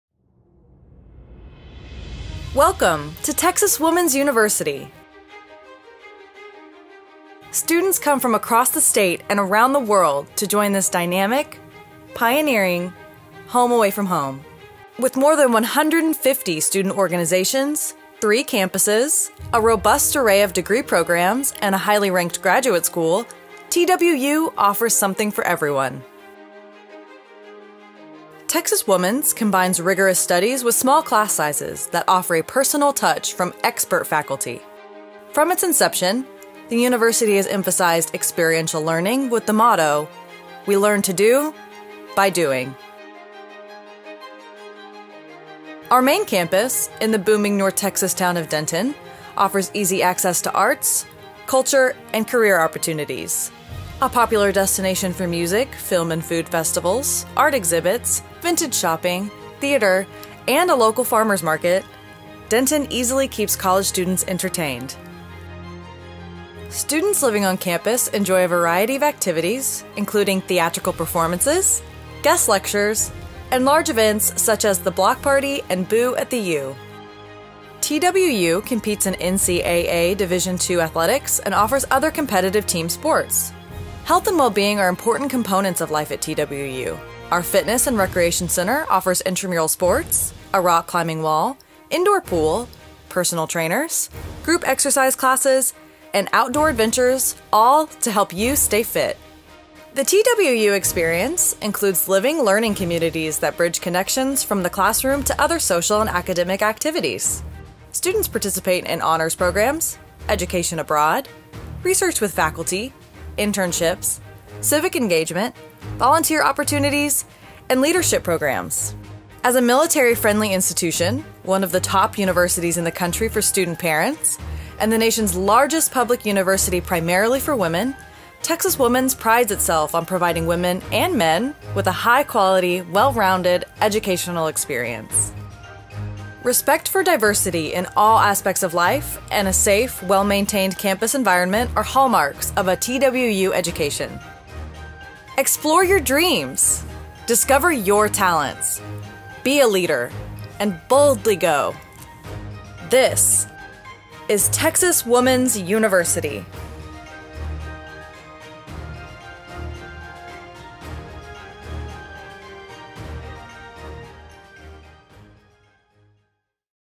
Welcome to TWU
Take a tour of Texas Woman's University and see for yourself how legacy, innovation and research collide to provide a unique perspective and environment that enables our students to Boldly Go!